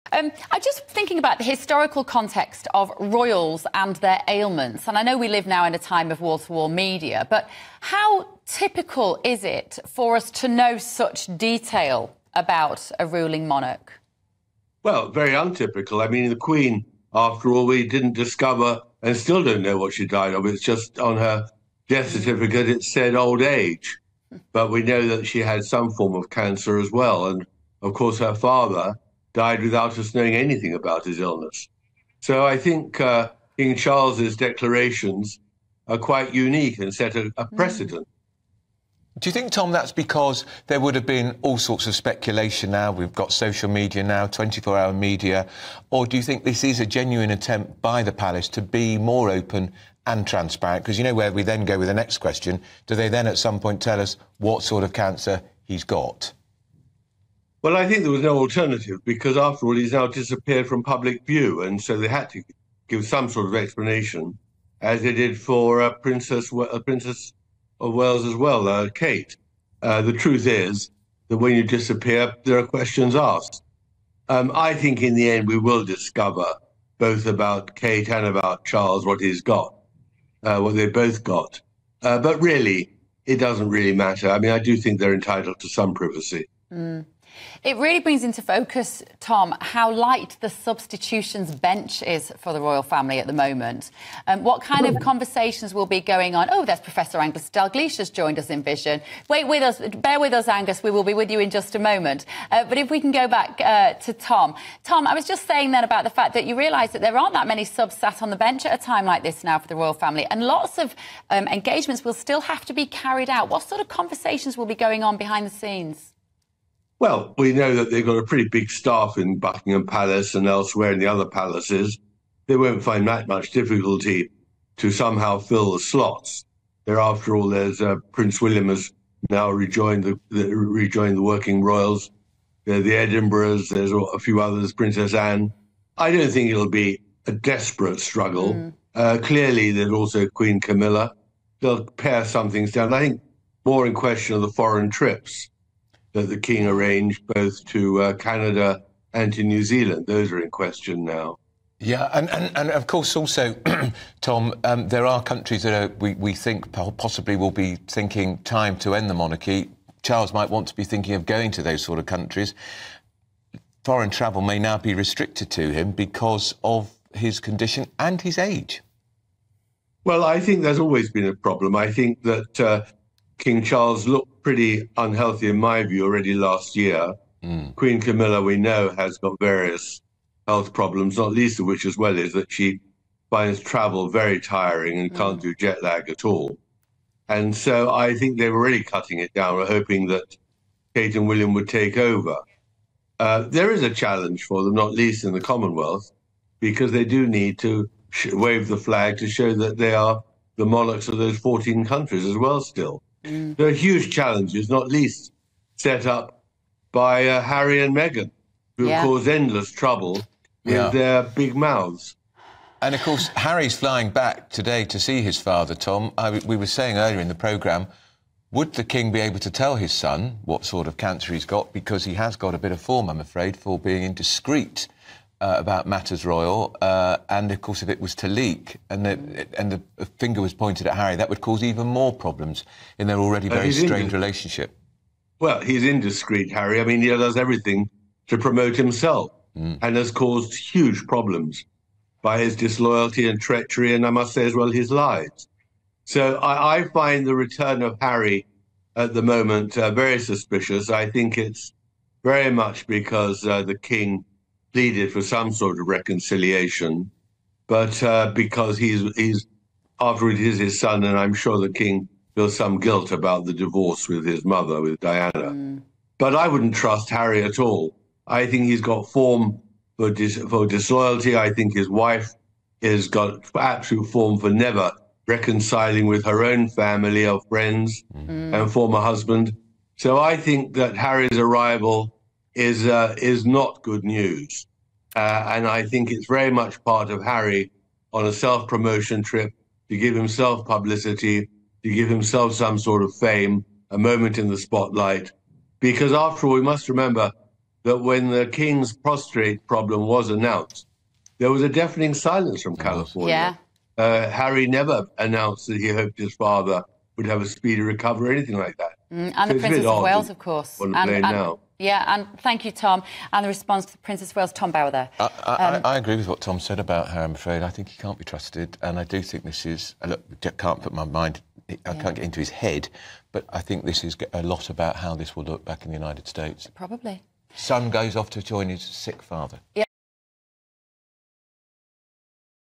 King should be 'suspicious' of Prince Harry 'PROMOTING HIMSELF' as he returns after cancer diagnosis
Duke of Sussex 'does everything to promote himself… He has form for disloyalty’.

Royal Commentator Tom Bower says King Charles should be ‘suspicious’ of Prince Harry as he returns to the UK following the King’s cancer diagnosis.